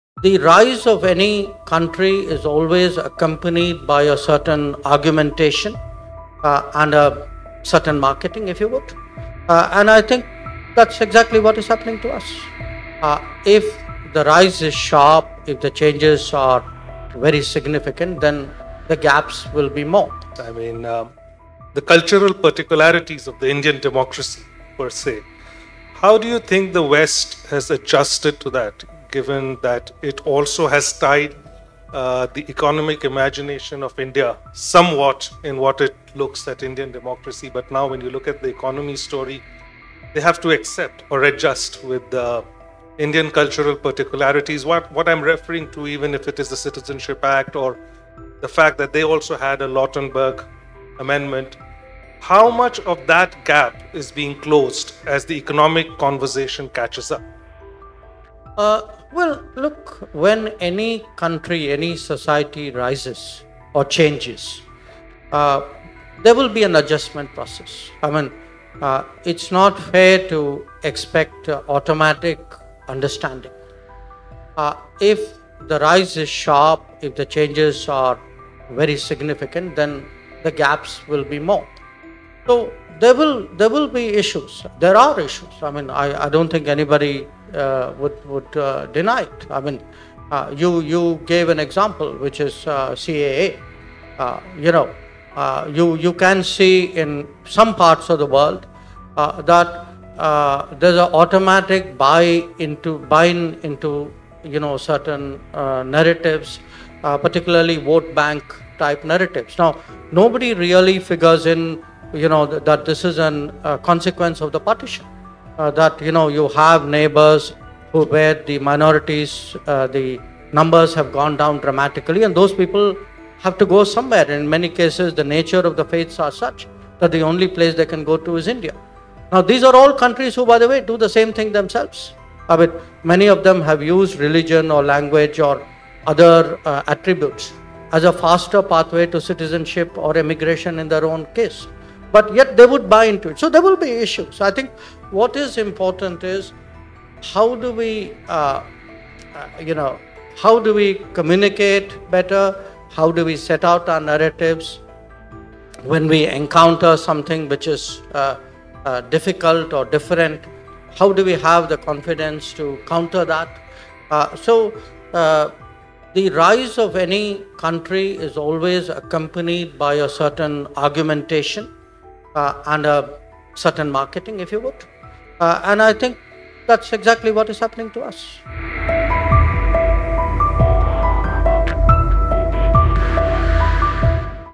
Jaishankar defends CAA, says Western countries themselves have used similar policies for immigration
During a candid conversation with Pranab Dhal Samanta from The Economic Times at the ET Awards 2023, External Affairs Minister Dr S. Jaishankar touched upon the global response to the CAA. He defended the policy, stating that it was unfair for Western nations to criticize it when they had implemented similar immigration policies in the past.